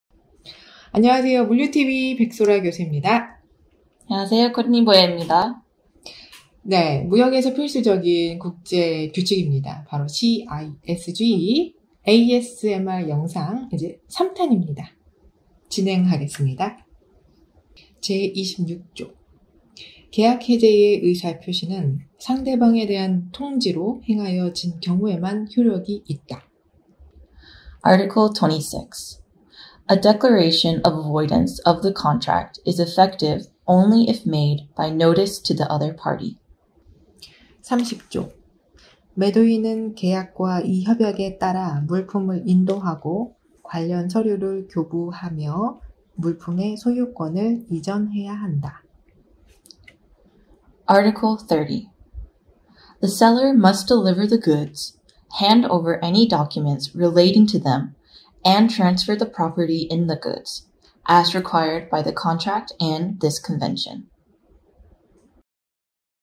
( 백소라의 무역 코칭 ) 🇰🇷 🇺🇸 무역 CISG ASMR ep.3 미리보기
무역 CISG ASMR ep.3 편 부터는 1분 미리보기 영상으로 올라옵니다

출연진  : 백소라 , Courtney Boyett
편집 : 정PD

물류TV
백소라 교수,  유통물류학과 학과장

혹시나 보고싶거나, 유익한 컨텐츠가 있다면 댓글로 남겨주세요.